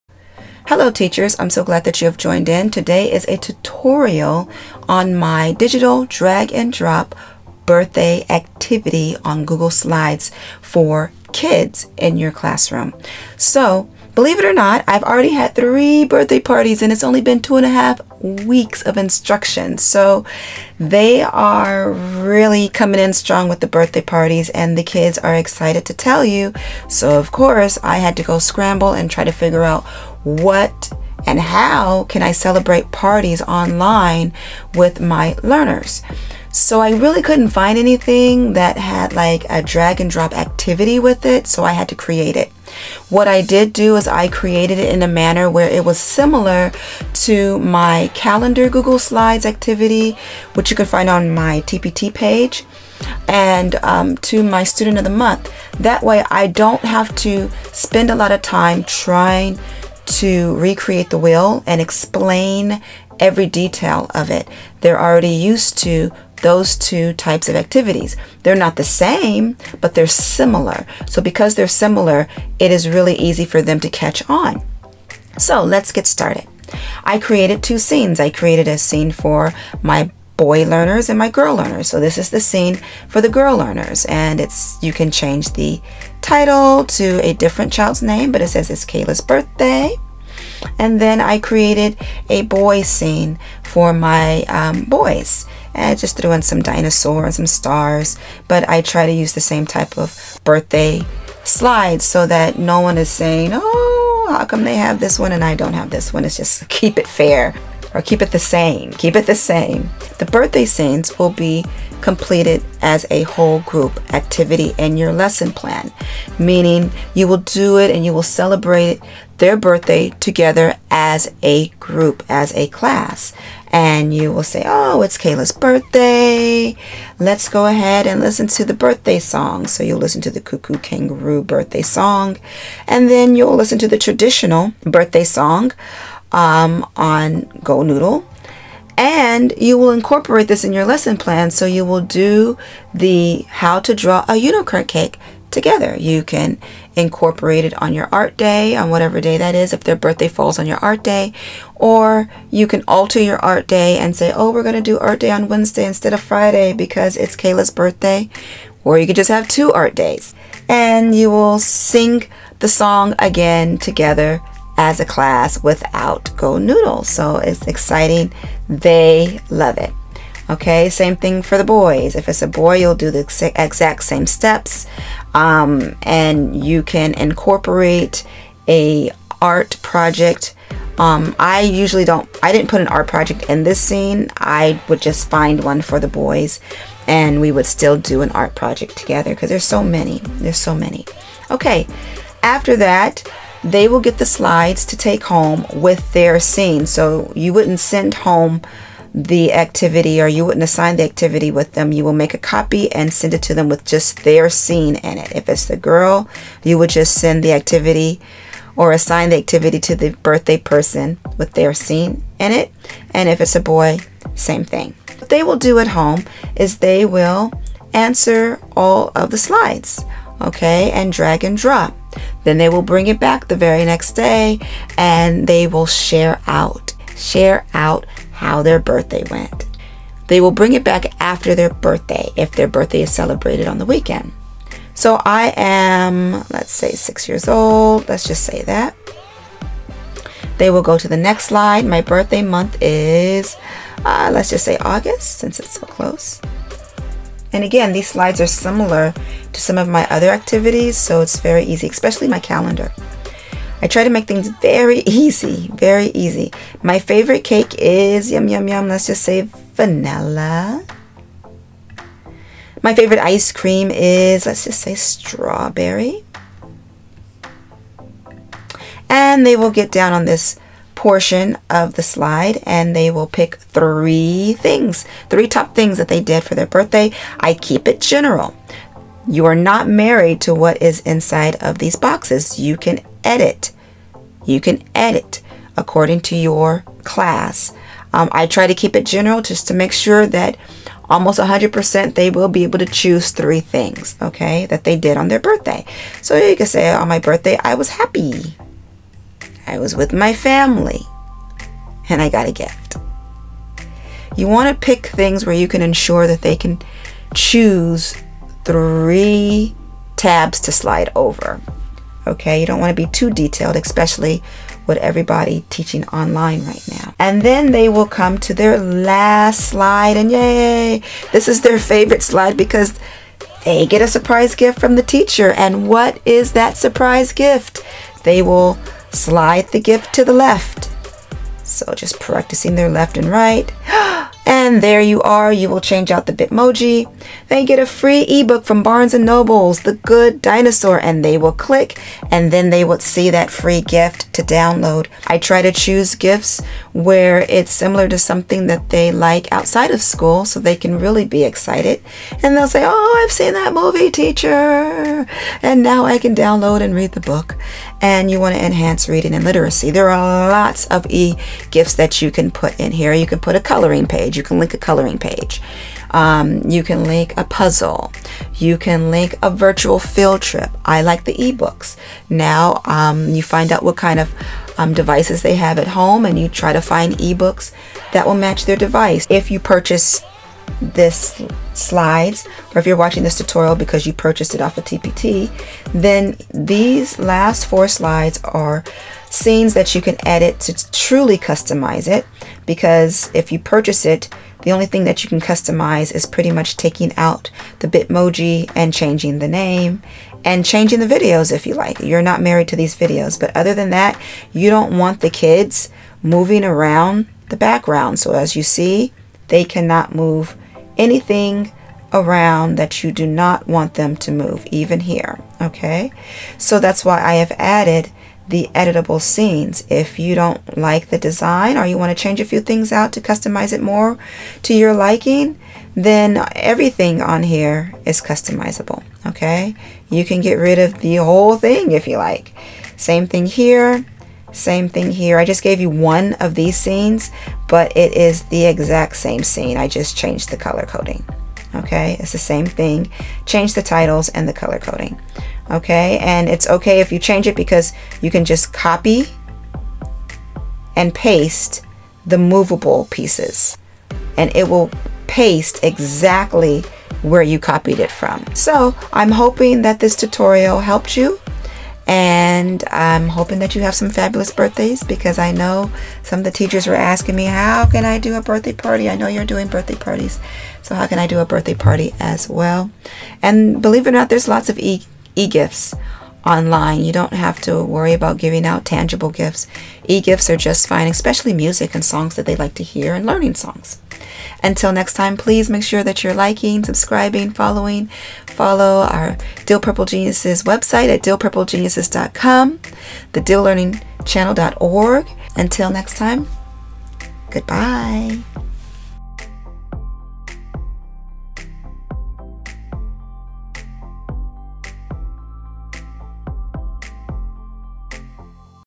Google Slides Drag and Drop Happy Birthday Spotlight| Tutorial For Teachers
Example  and Tutorial for Teachers to see to recreate their own version - Teachers can use this Google Slides to spotlight learners on their birthday.  This is an activity that can be completed as a whole group and individual celebration. It even comes with a teacher surprise e-gift in the slides and an art activity. Make it a true celebration by incorporating it into the lesson plan.

Visit the Dill Purple Geniuses TPT Store to download a copy@